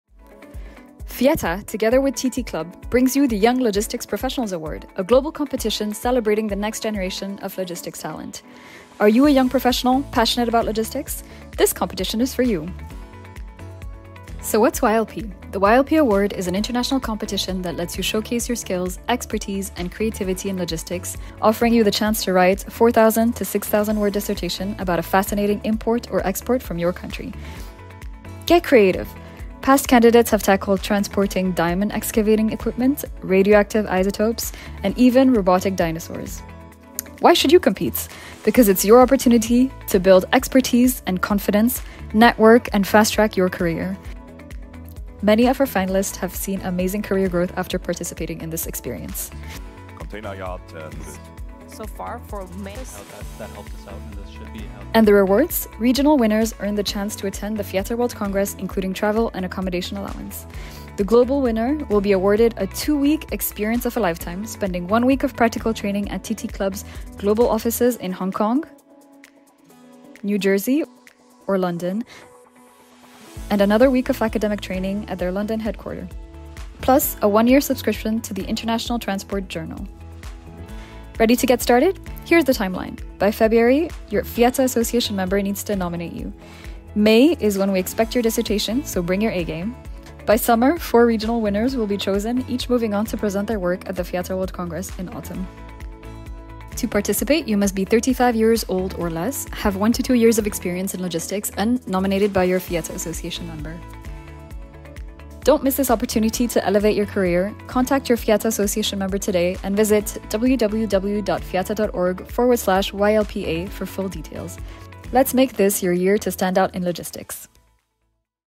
Young Logistics Professionals (YLP) Award - Promotional Video
FIATA and TT Club present the YLP Award, with this video, you will familiarise yourself with the competition, its goal and requirements to participate!